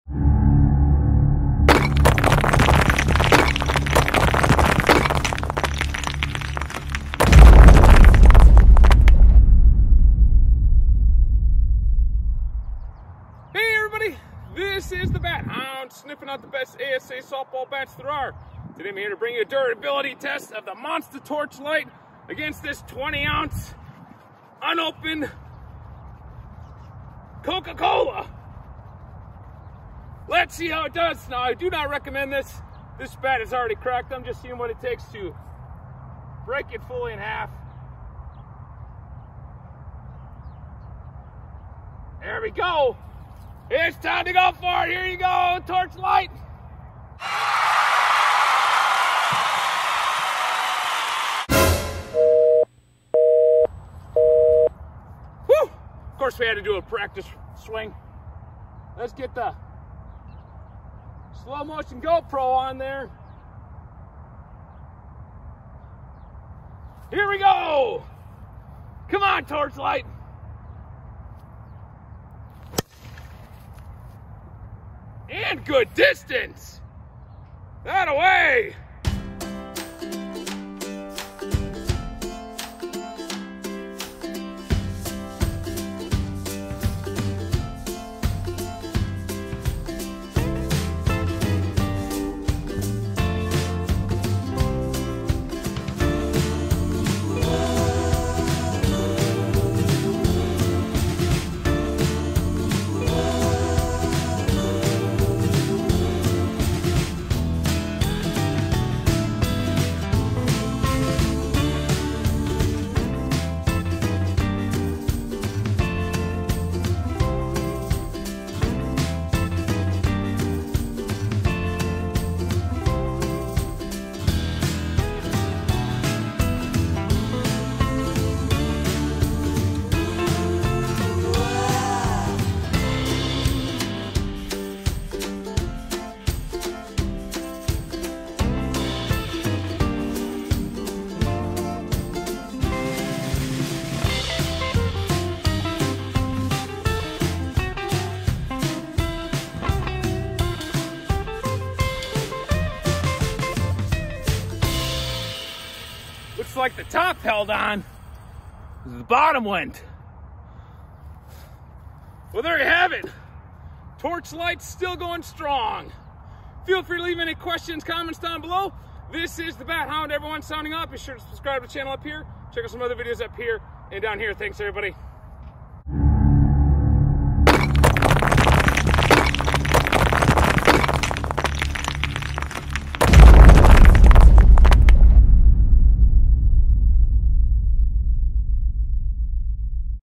Softball bat vs. Unopened 20oz bottle of soda (BAT VS. RANDOM OBJECTS)
Today we see the Softball bat vs. Unopened 20oz bottle of soda, as part of the Bat vs. Random Objects series.
POP TOSS GREEN PITCHING MACHINE